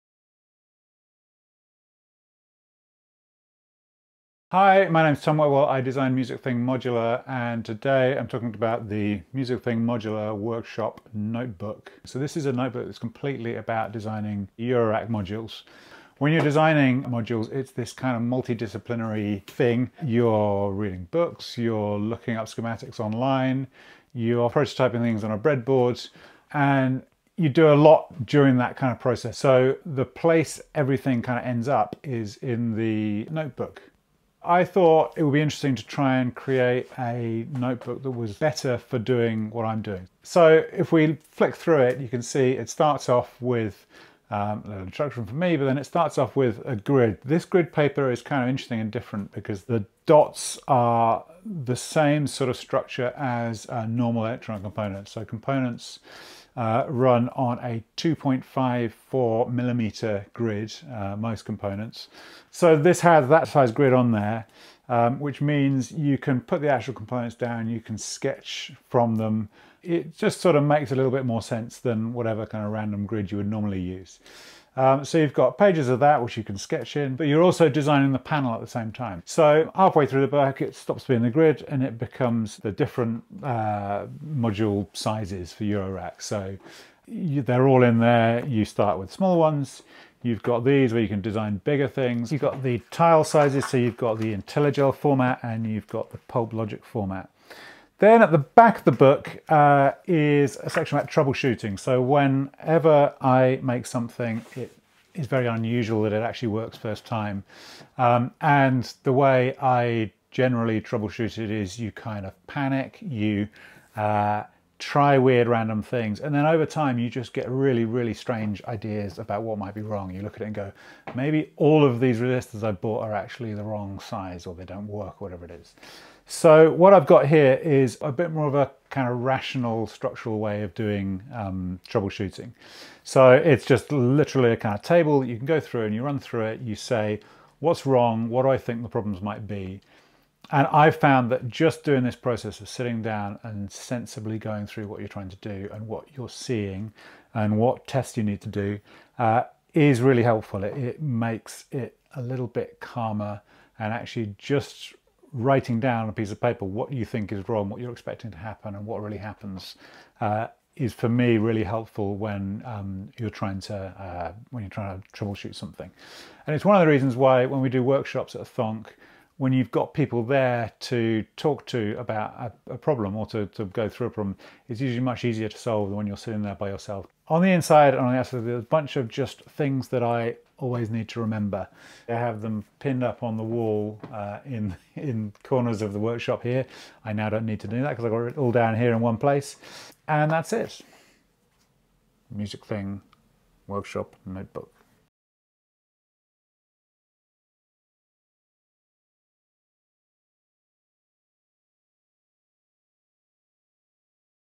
Music Thing Modular - Workshop Notebook
The Music Thing Modular Workshop Notebook is a 100 page ring-bound A5 notebook made for designing electronics, in particular Eurorack synth modules. Includes free sticker. INCLUDES CODE TO GET 10 GBP OFF ANY MUSIC THING FULL KIT IN A FUTURE ORDER!